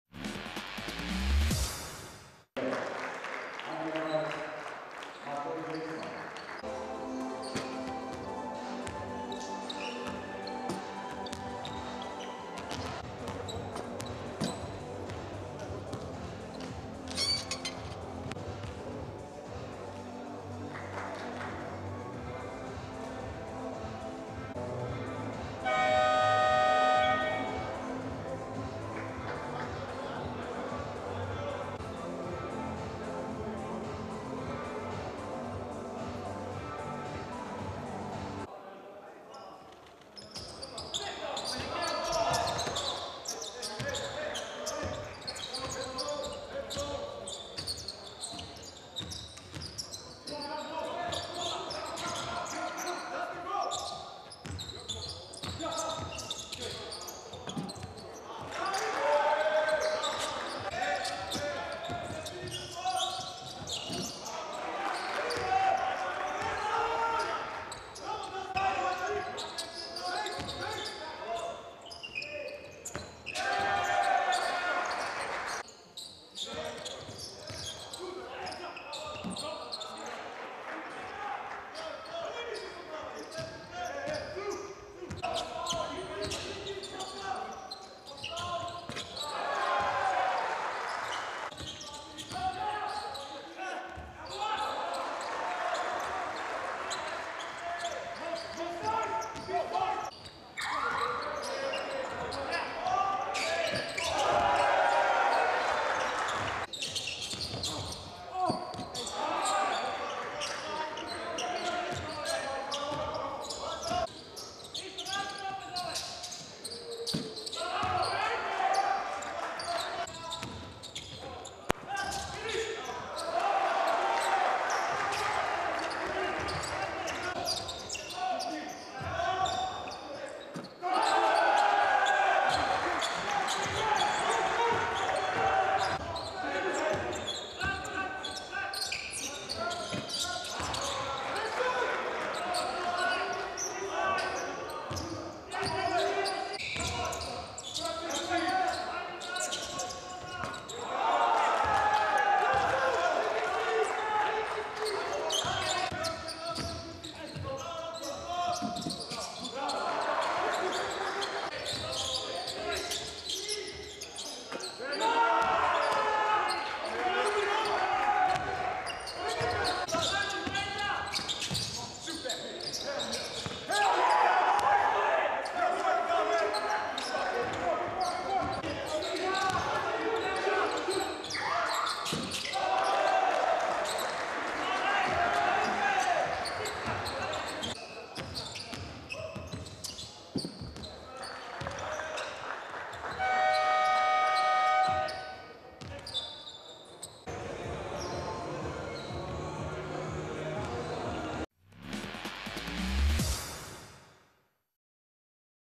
ABA League 2 2021/22 highlights, Round 4: Vrijednosnice Osijek - MZT Skopje Aerodrom (12.11.2021)
Take a look at the highlights of the 2021/22 season ABA League 2 game between Vrijednosnice Osijek and MZT Skopje Aerodrom in Osijek.